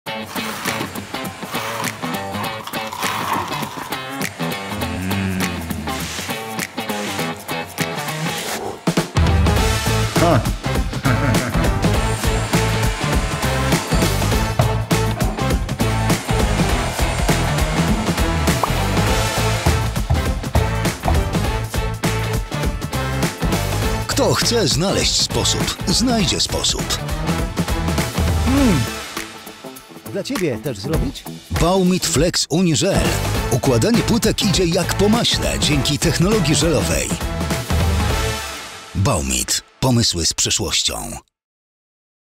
Baumit FlexUni Gel - układanie płytek idzie jak po maśle!
Układanie płytek to spore wyzwanie, dlatego warto się do niego dobrze przygotować. Chcesz, aby praca poszła jak po maśle? Wypróbuj elastyczną zaprawę klejową do płytek FlexUni Gel od Baumit! Żelowa technologia sprawia, że produkt jest wytrzymały, posiada regulowaną konsystencję i gwarantuje łatwą i szybką aplikację. Kładzenie płytek z Baumit FlexUni Gel to bułka z masłem! Sprawdź szczegóły na naszej stronie